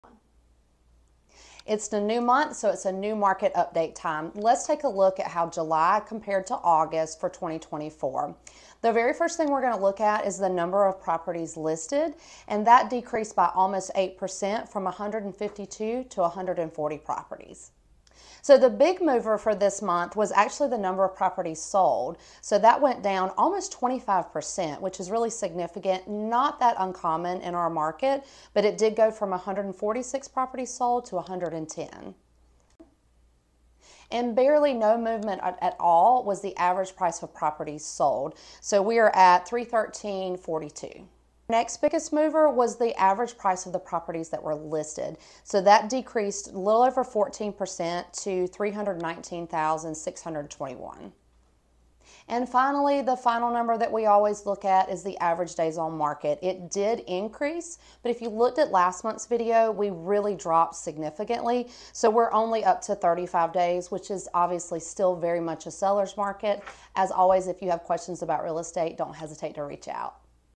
August 2024 Market Update
It's September, let's check out how August's real estate performed compared to July's. It's still a great market for sellers, let's take a look and see what’s new!

✔️Listings are down by 7.9%
✔️Properties sold are down by 24.7%
✔️The average list price experienced a drop of 14.4%, now at $319621
✔️Average days on the market for almost 35% from 26 to 35.

All this information is sourced from the GAMLS, ensuring accurate and up-to-date market insights. Like July, it is still a great time for sellers! If you'd like to discuss how this market might affect your real estate goals reach out today!

Jessica Herrmann, Broker - Great GA Realty